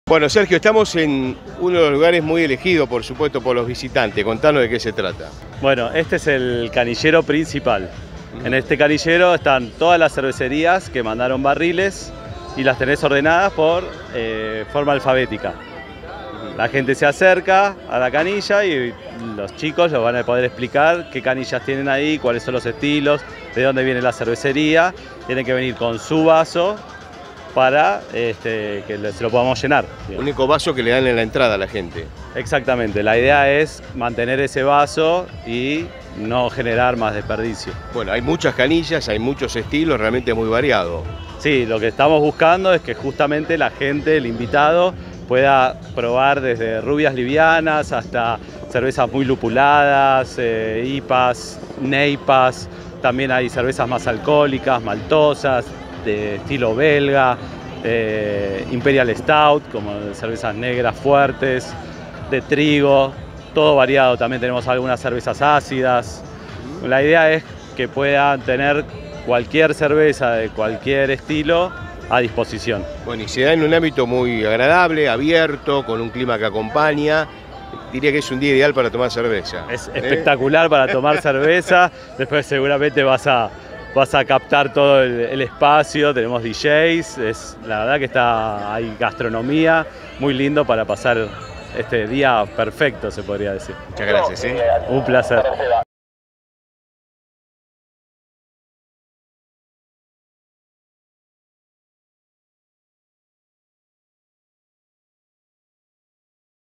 EL PATIO CERVECERO EN CERVEZAR
EL PROVINCIAL dialogó con Sergio, uno de los canilleros en el predio de la Rural de Palermo en CERVEZAR, el Festival Internacional de la Cerveza Artesanal.

La Cámara de Cerveceros Artesanales de Argentina presentó la primera edición del Festival internacional de la Cerveza Artesanal cuyo principal objetivo fue crear espacios para que el público en general tenga acceso y pueda conocer todos los secretos de la cerveza artesanal en donde los productores, bares, empresas y marcas en general tuvieron un feedback y contacto directo con toda la comunidad cervecera del país y Latinoamérica a través de experiencias exclusivas.

En la Argentina, hay una enorme variedad de cervezas artesanales para probar y descubrir. Cada productor se esfuerza por crear una cerveza única con ingredientes nobles y particulares, pensados para que la experiencia, en el bar o en tu casa sean completas.

Con la entrada le entregaban un vaso para ir directamente al patio cervecero y disfrutar de cada una de las fábricas y cervecerías que presentaron sus productos.